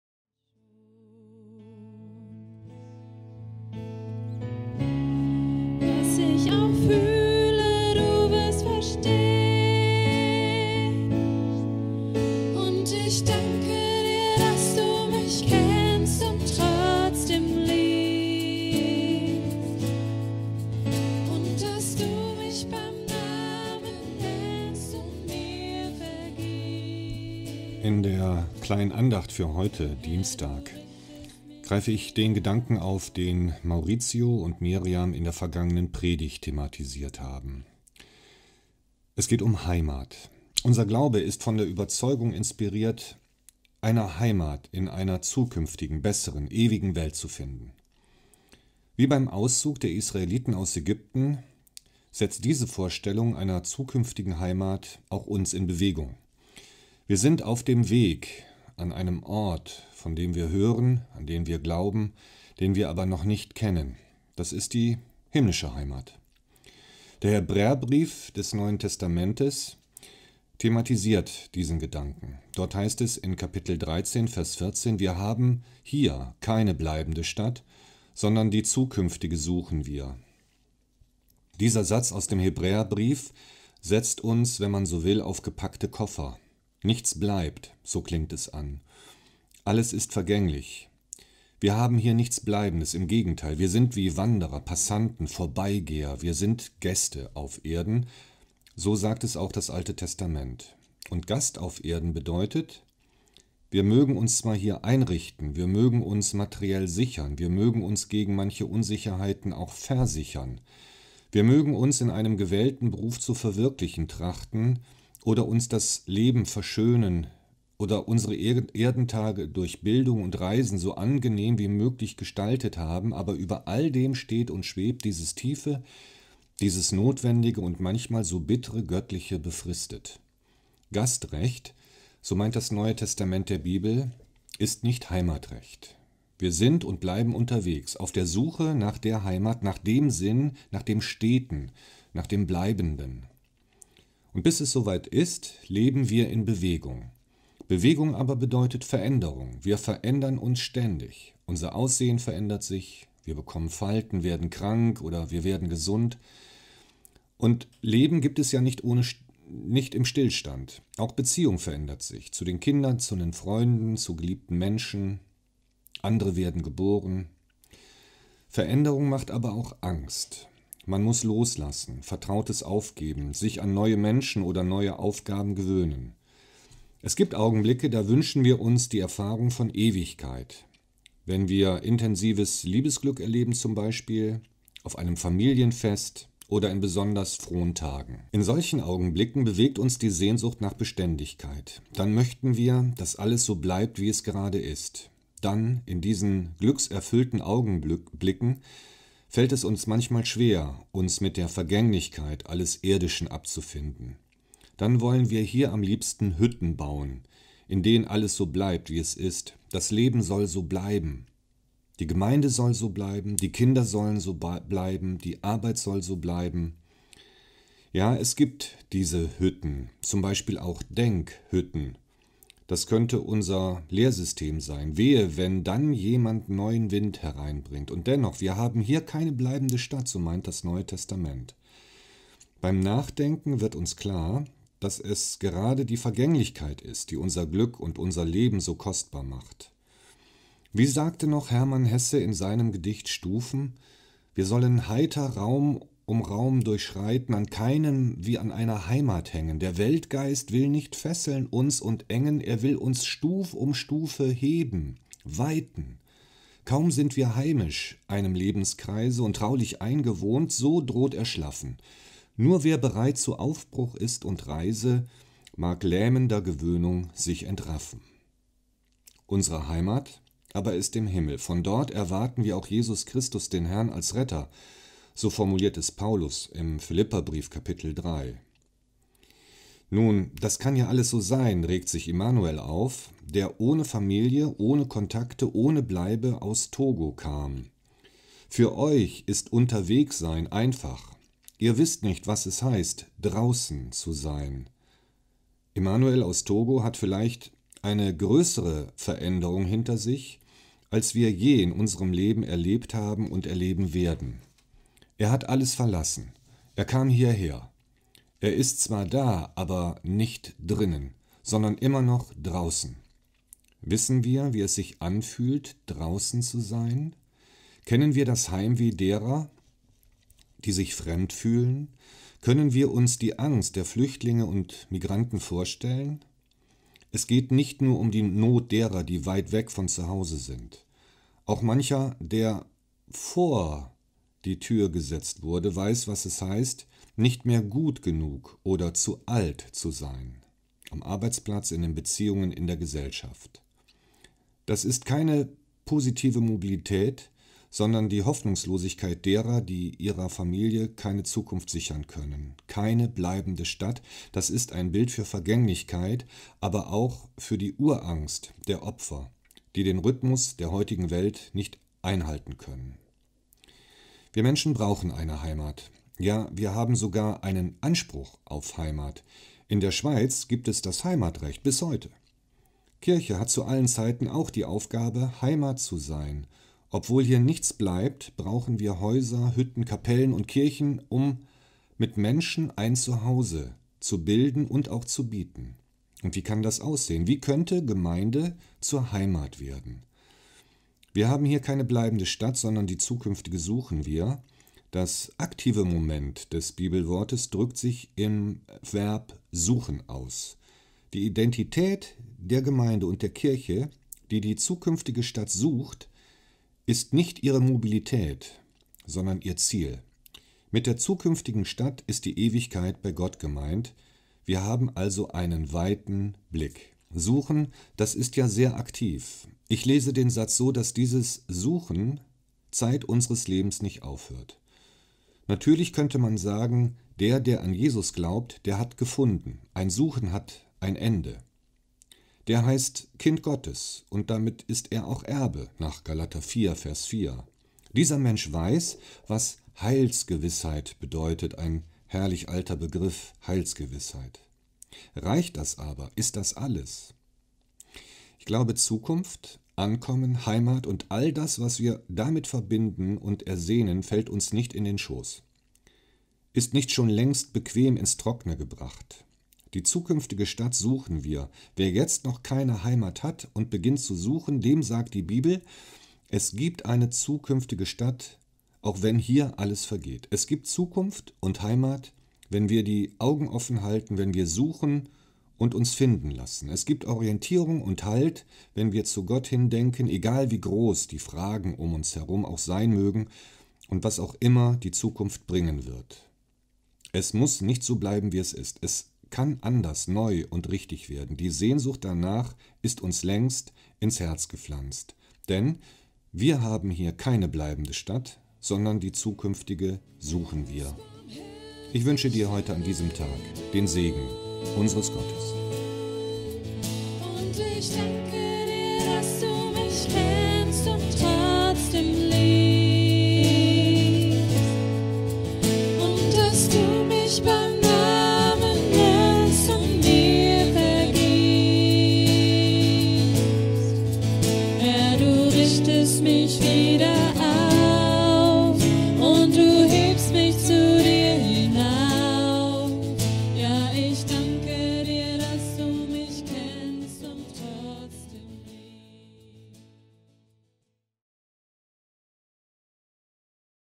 Heimat | Andacht | Oncken Kirche Hamburg
Ein kurzer Impuls von Pastor Frank-Eric Müller zum Thema Heimat.

Photo by Ansgar Scheffold on Unsplash